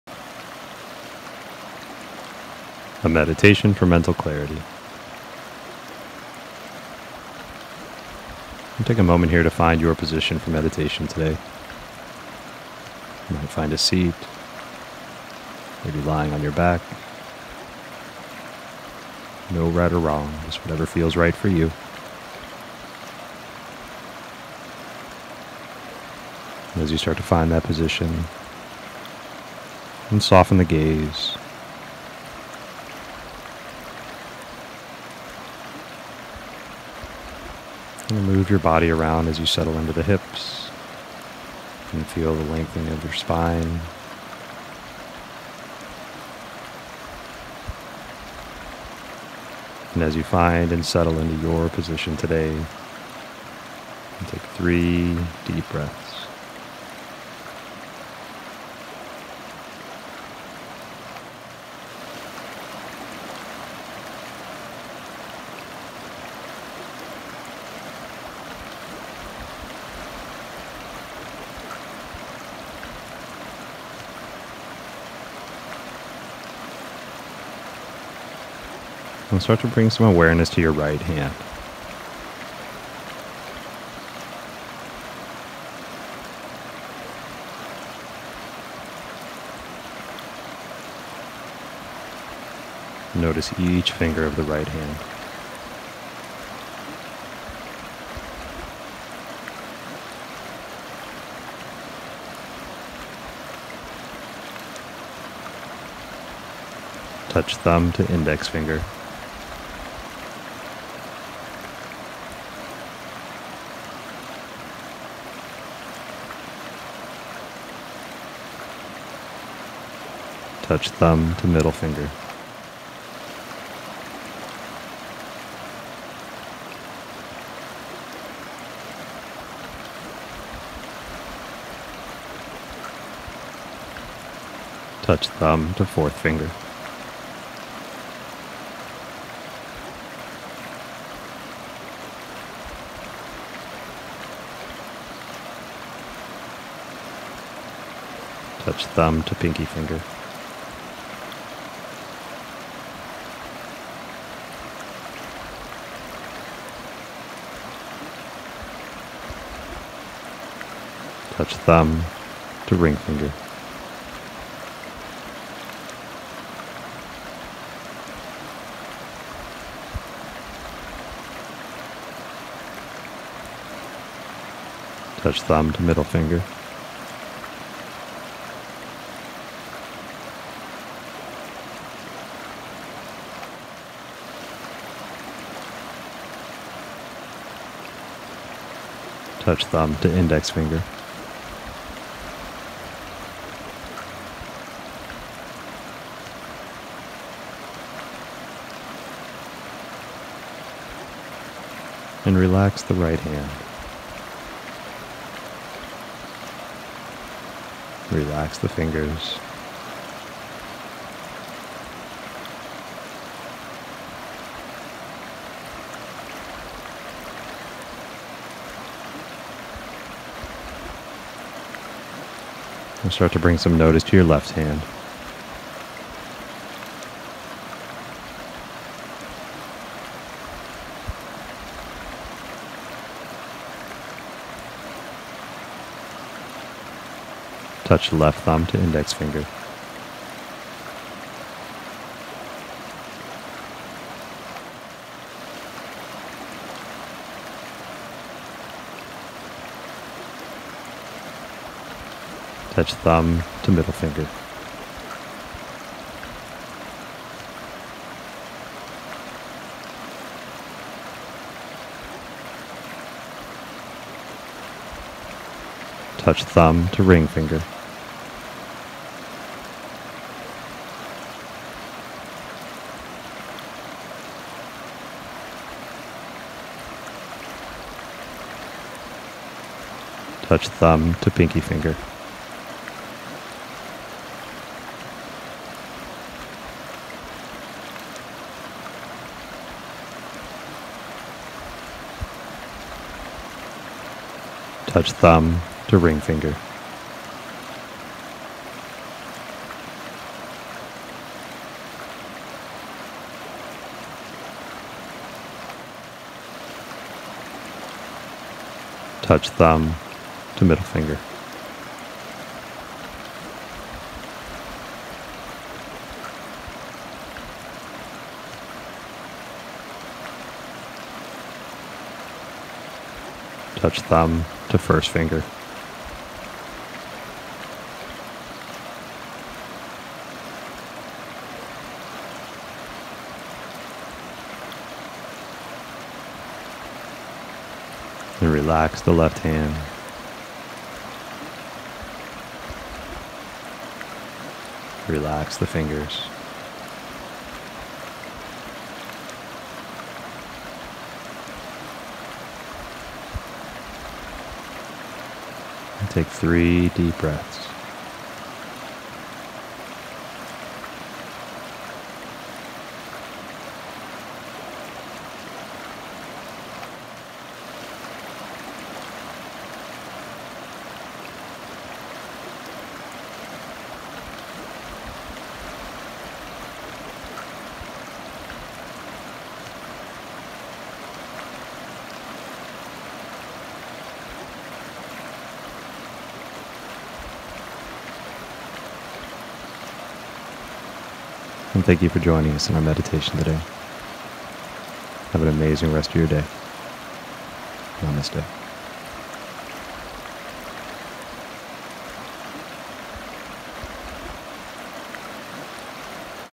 Meditation for Mental Clarity I Adam Riehl
7 Minute mediation for mental clarity.